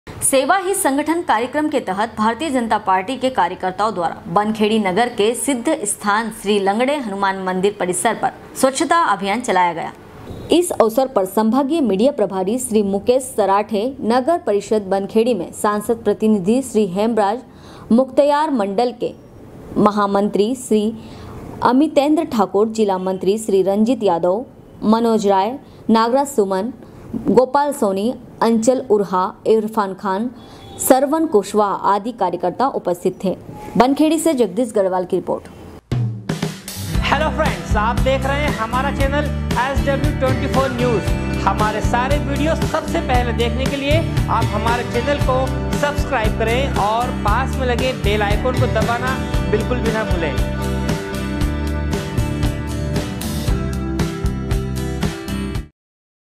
SW 24 News Madhyapradesh
sevahisangthan bankhedhi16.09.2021 सेवा ही संगठन कार्यक्रम के तहत भारतीय जनता पार्टी के कार्यकर्ताओं द्वारा बनखेड़ी नगर के सिद्ध स्थान श्री लंगड़े हनुमान मंदिर परिसर पर स्वच्छता अभियान चलाया गया